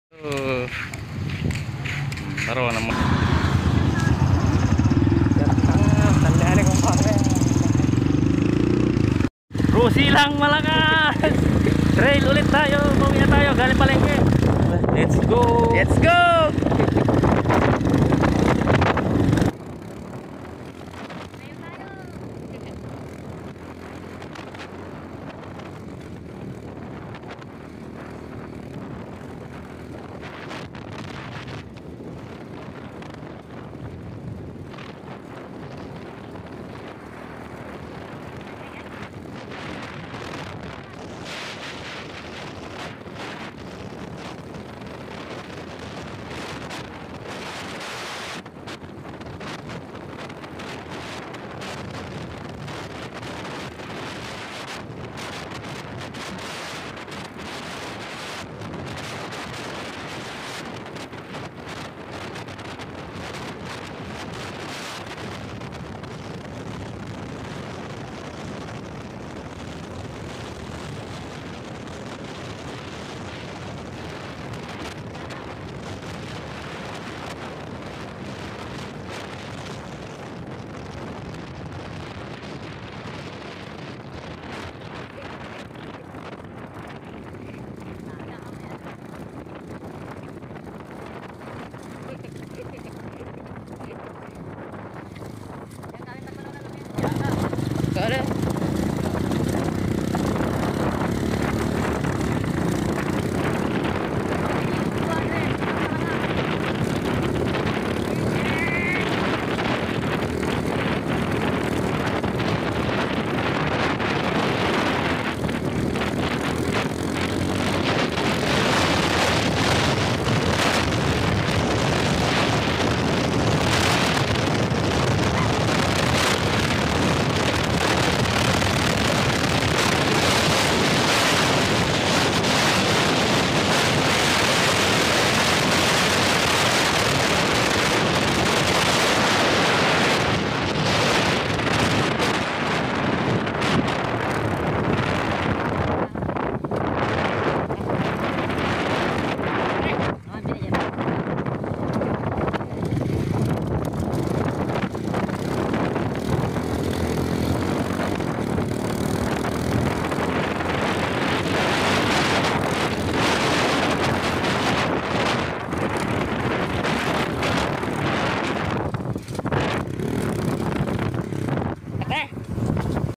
KRZ200 lakas neto pang trail
krz200 sinubukan mag trail
pauwi gaming palengke